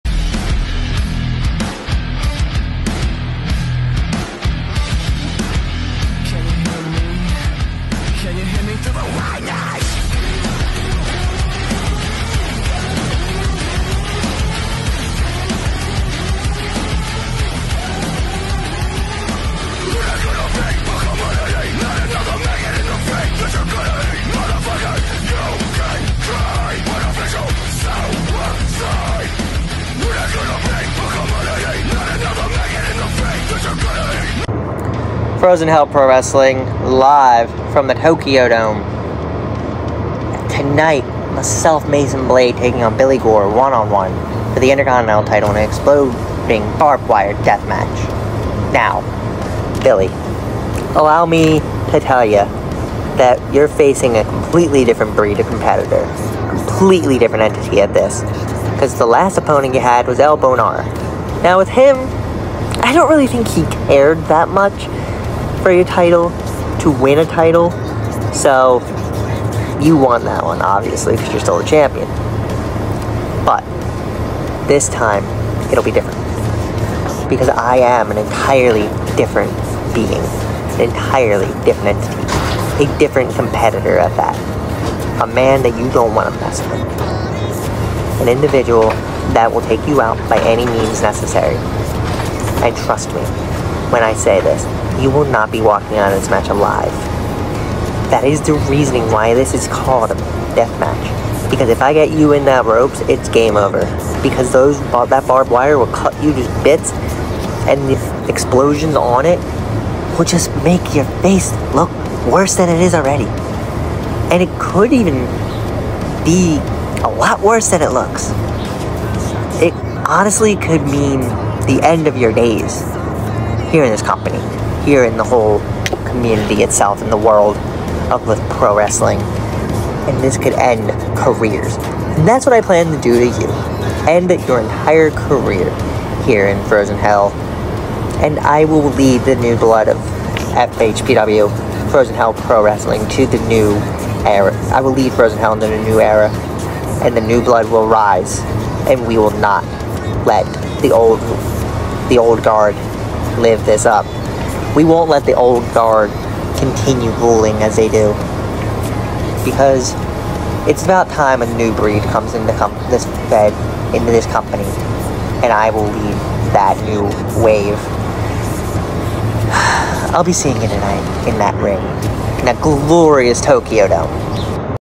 FHPW 8-27-23 Mason Blade Vs Billiy Gore IC title
Mason Blade takes on Billy Gore for the FHPW Intercontinental Championship.